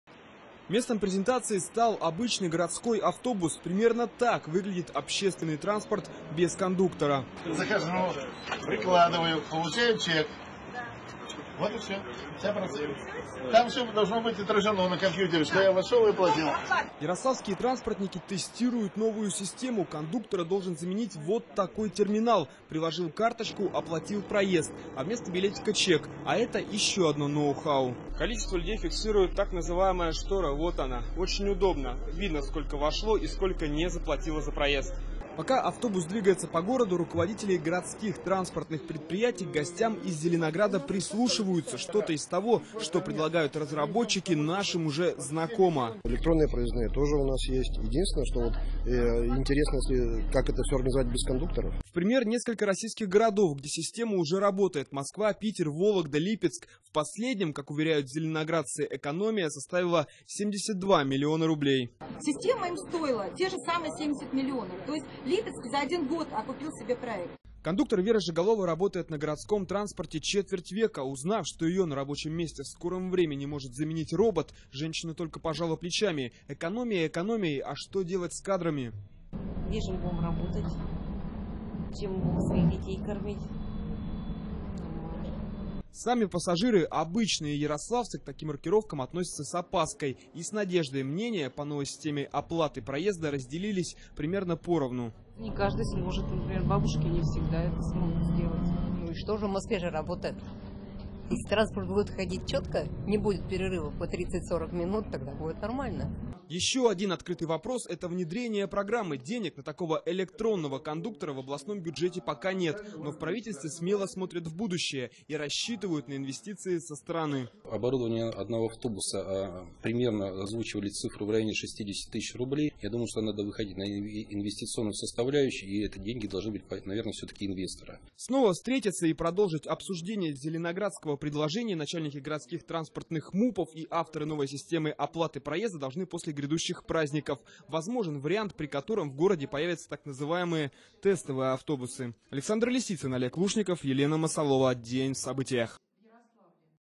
В Ярославле новая электронная система может заменить кондукторов в общественном транспорте
Без кондукторов. В Ярославской области может появиться новая система оплаты проезда в общественном транспорте. Сегодня в наш город приехали разработчики этого проекта из Зеленограда.